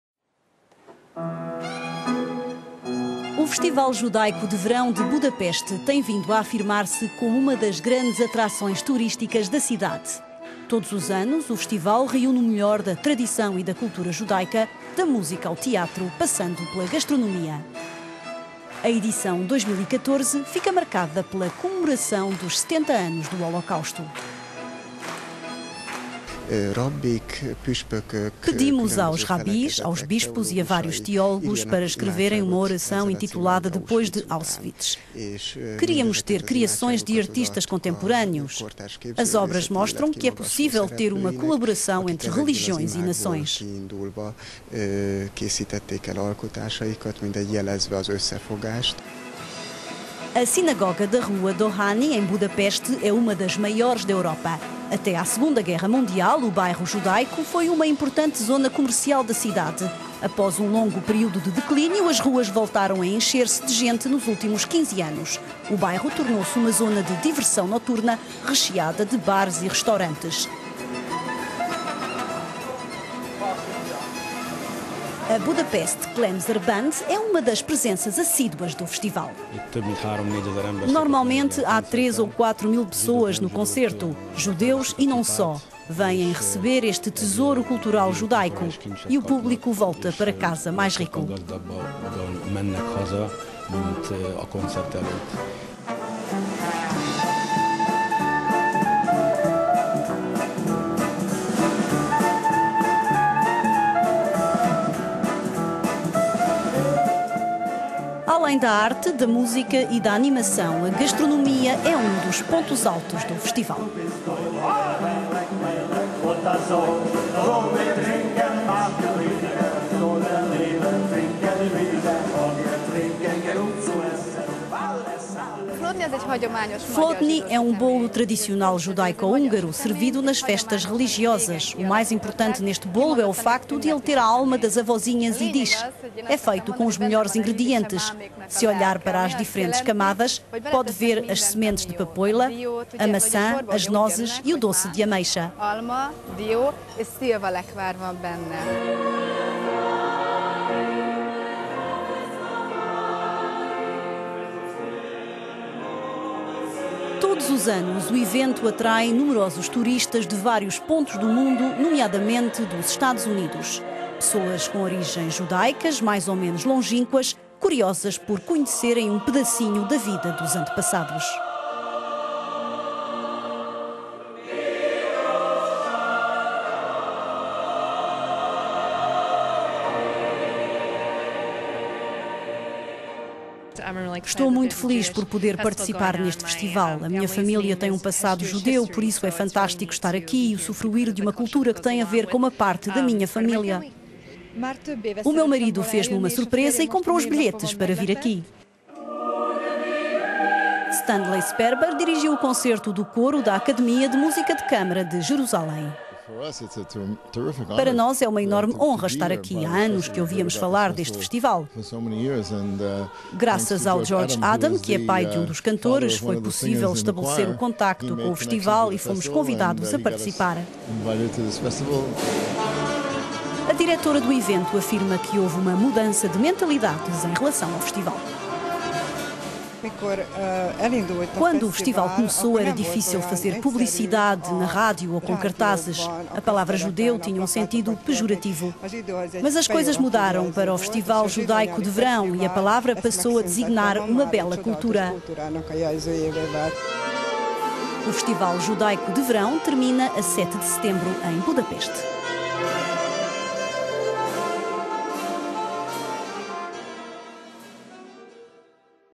Turistas enchem ruas de Budapeste durante Festival Judaico de Verão - le mag
O Festival Judaico de Verão de Budapeste tem vindo a afirmar-se com uma das grandes atrações turísticas da cidade.

Todos os anos, o festival reúne o melhor da tradição e da cultura judaica, da Música às artes plásticas, passando pela gastronomia.

A edição 2014 fica marcada pela comemoração dos 70 anos do holocausto. A data é assinalada com uma exposição na principal sinagoga da cidade.

"Pedimos aos rabis, aos bispos e a vários teólogos para escrever uma oração intitulada 'Depois de Auschwitz…